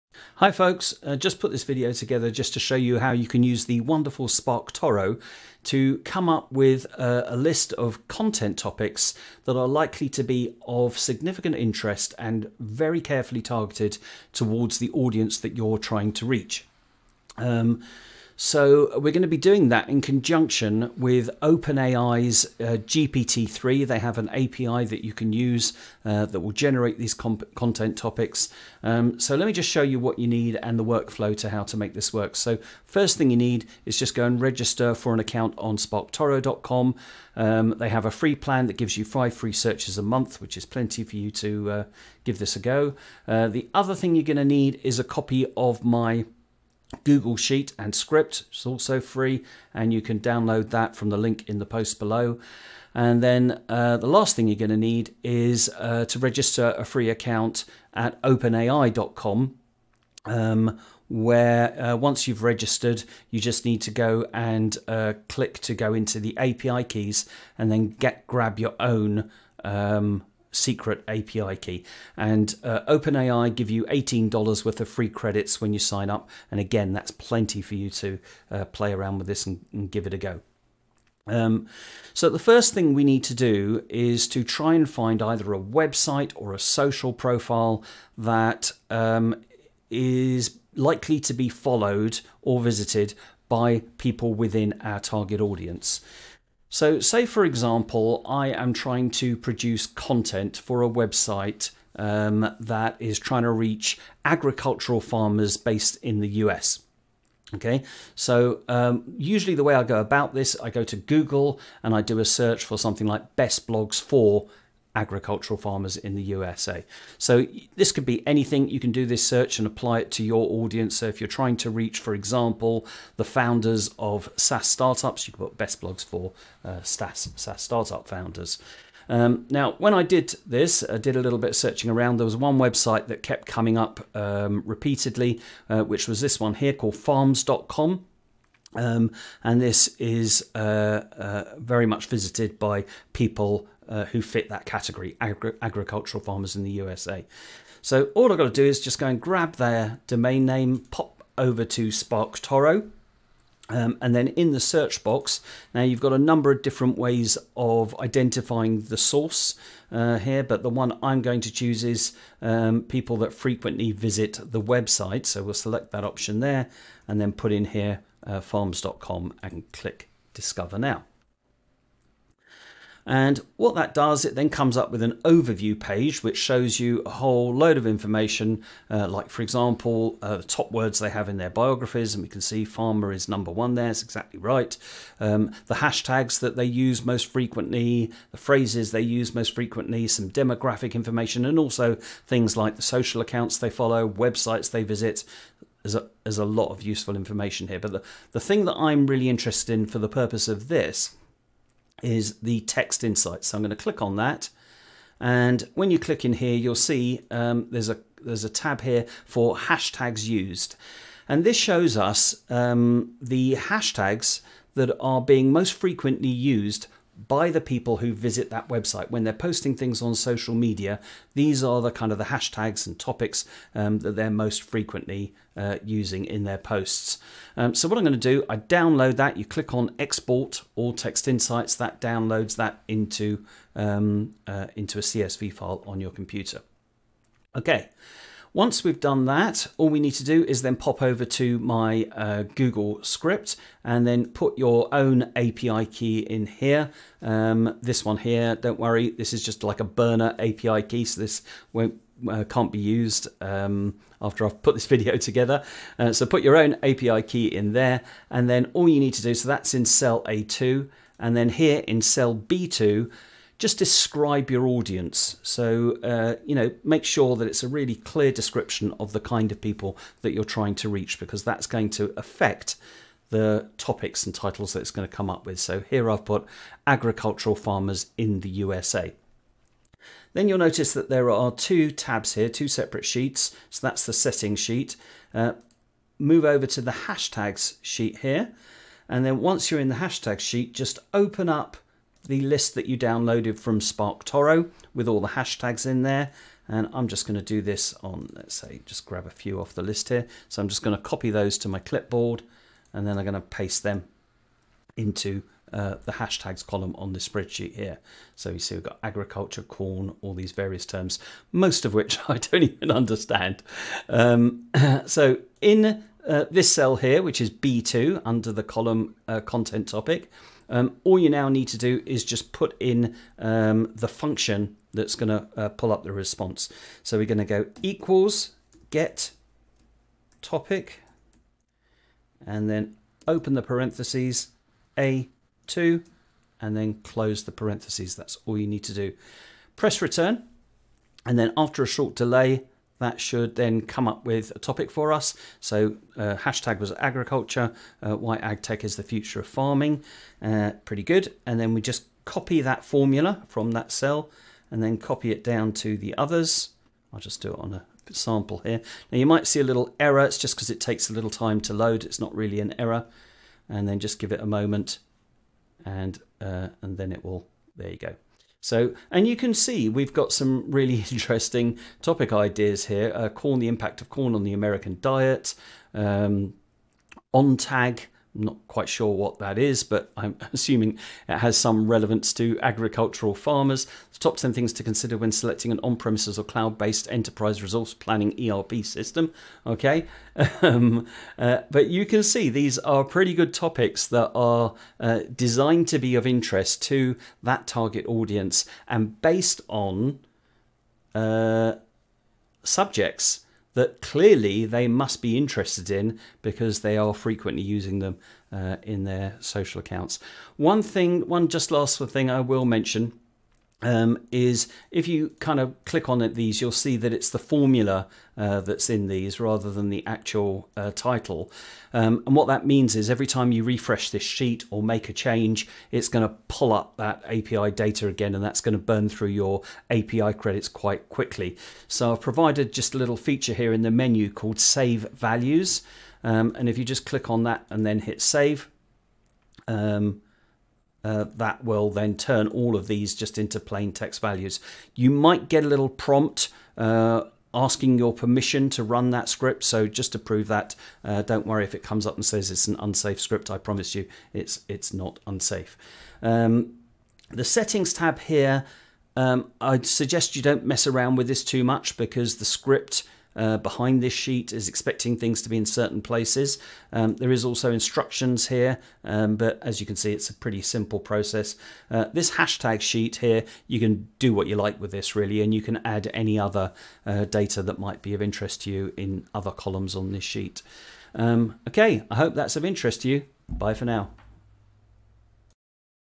How to use GPT-3 to generate content topics based on the hashtags used by your target audience.
Use this free Google Sheets and GPT3's API to generate content topics based on the hashtags used by your target audience.